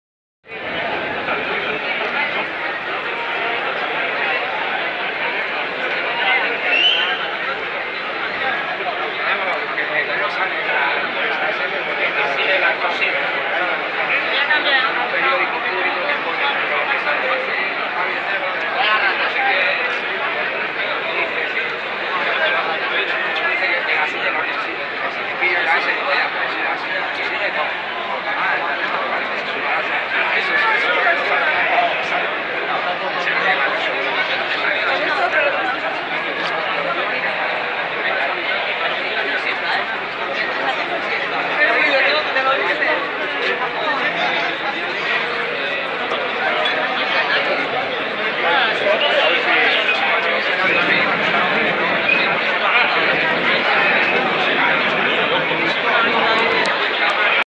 MOSAICO de ESKORBUTO
Inauguración del mosaico dedicado al grupo Punk Eskorbuto en Mamariga (Santurce).